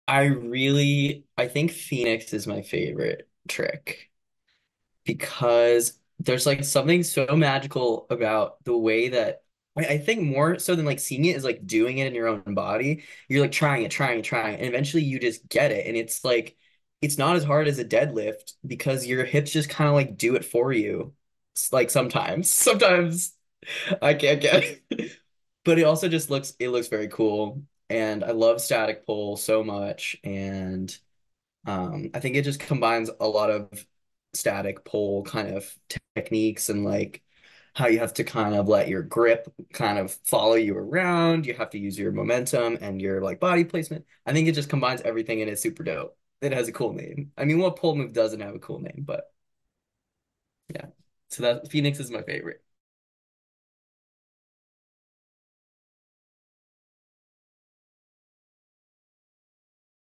Connor Jocktober's Favorite Pole Trick is Phoenix!
We were so surprised to learn that pdphoenix is @connor.jocktober ‘s favorite pole trick!

Is Phoenix your favorite? 🐦‍🔥

If it isn’t, check out the tips Connor gave us to get into this trick in our new podcast episode out now!

Get all the links to listen & watch via poleonthecall.com and share with a friend who may enjoy!